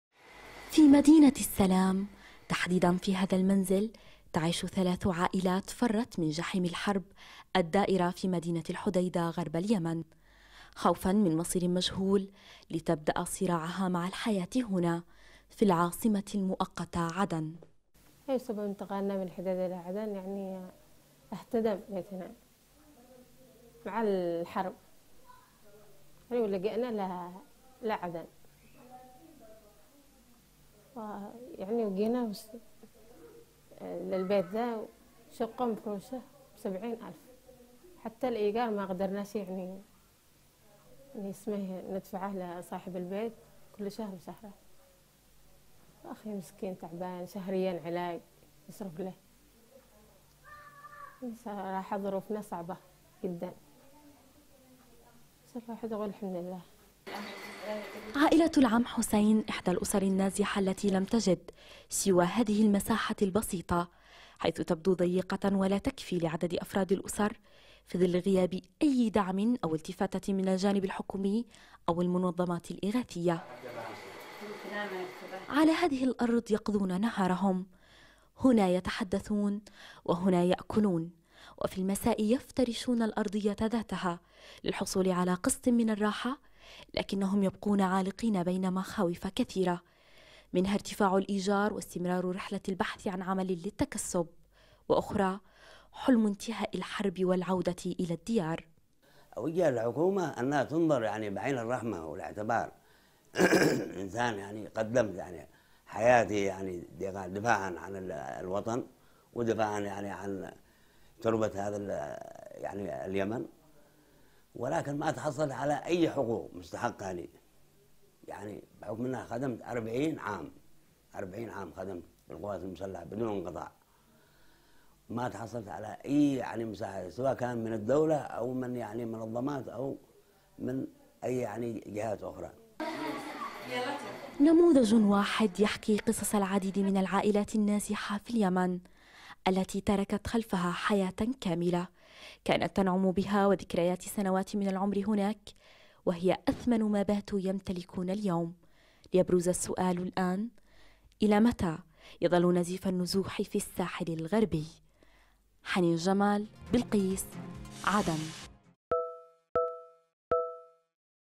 عدن ثلاث عائلات نازحة من الحديدة تشكو تجاهل الحكومة والمنظمات الإغاثية لمأساتها|تقرير: حنين جمال
في عدن.. تعيش ثلاث عائلات فرت من المعارك التي تشهدها مدينة الحديدة ظروفاً معيشية صعبة.
وتشكو العائلات من تجاهل الجهات الحكومية، والمنظمات الإغاثية، وعدم تقديمها أية مساعدات أو معونات...